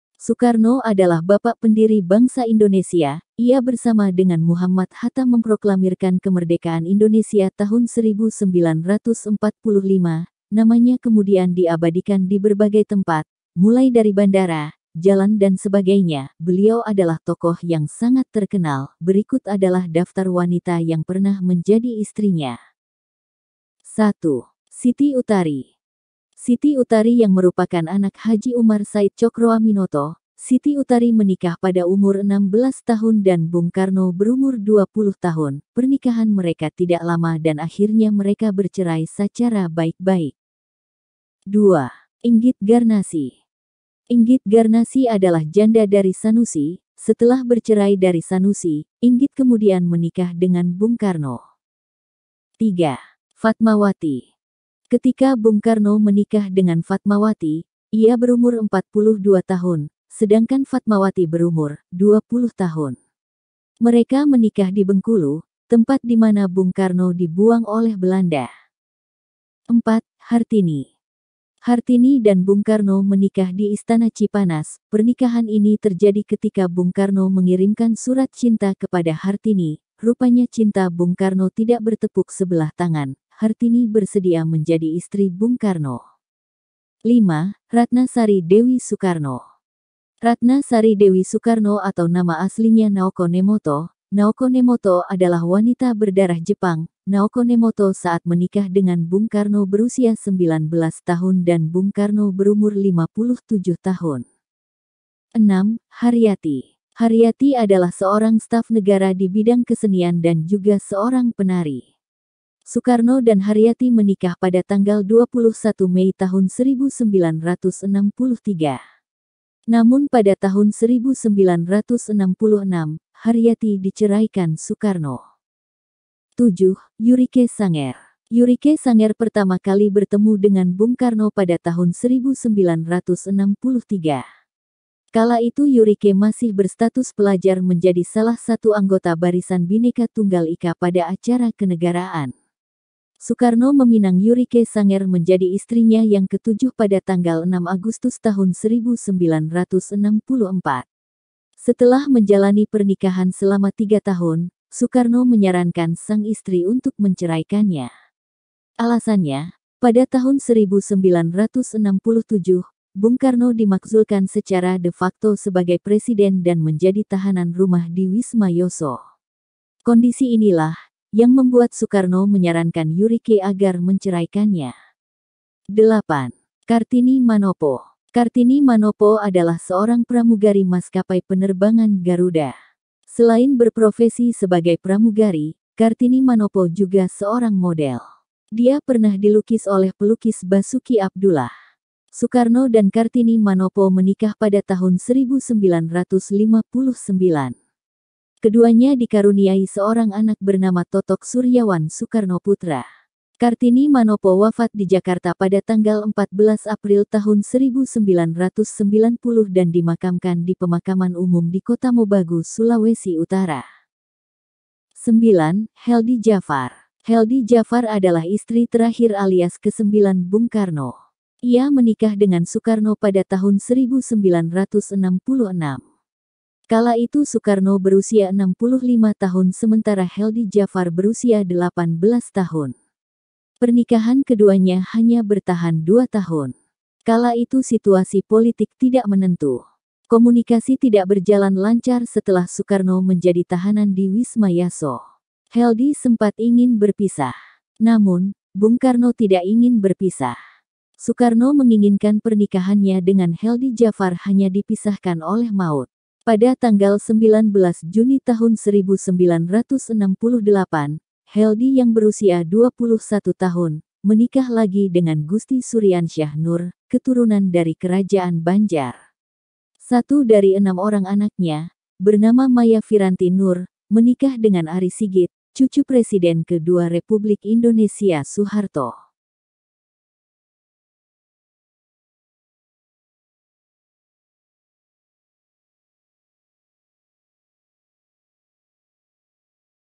Daftar Wanita (Ibu Negara) Yang Pernah Menanpinggi Presiden Soekarno
Soekarno atau Bung Karno dikenal sebagai sosok laki-laki yang terkenal akan karismanya dan penuh wibawa. Bukan hanya itu, bapak proklamator Indonesia ini juga berhasil memikat perhatian banyak wanita.
Namun, sebagian masyarakat Indonesia hanya tahu bahwa Ibu Fatmawati merupakan istri satu-satunya Bung Karno. Ibu Fatmawati sendiri merupakan istri ketiga Bung Karno. Rupanya, Bung Karno memiliki 9 istri. Lantas, siapa saja istri Bung Karno?
Melansir dari beberapa sumber, ada 9 istri Soekarno lo. silahkan nonton lengkap di video